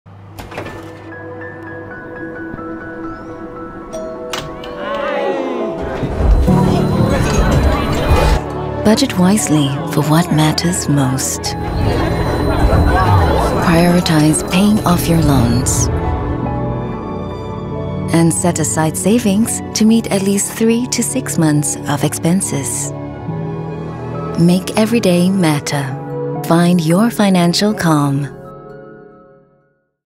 Budget wisely. Find your financial calm.
Managing your expenses and budgeting isn't just about numbers, it's about creating healthier financial boundaries.

Ensure you set aside at least 3 to 6 months worth of expenses for unforeseen circumstances.

Find your financial calm at cpf.gov.sg/BeReady